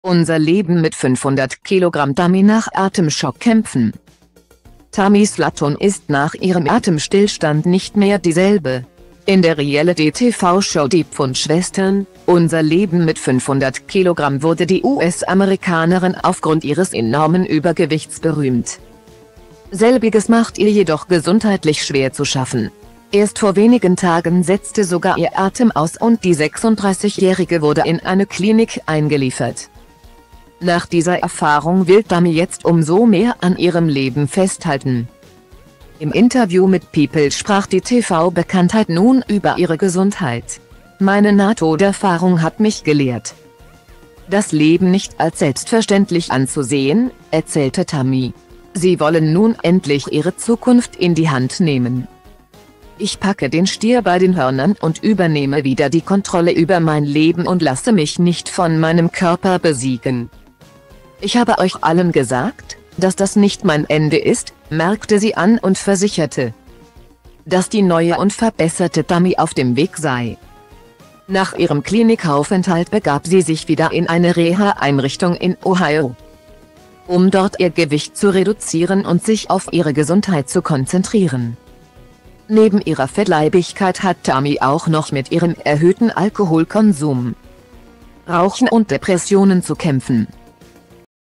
"Unser Leben mit 500 kg"-Tammy nach Atem-Schock kämpfen
Tammy Slaton ist nach ihrem Atemstillstand nicht mehr dieselbe! In der Reality-TV-Show "Die Pfund-Schwestern: Unser Leben mit 500 kg" wurde die US-Amerikanerin aufgrund ihres enormen Übergewichts berühmt.Selbiges macht ihr jedoch gesundheitlich schwer zu schaffen. Erst vor wenigen Tagen setzte sogar ihr Atem aus und die 36-Jährige wurde in eine Klinik eingeliefert.Nach dieser Erfahrung will Tammy jetzt umso mehr an ihrem Leben festhalten!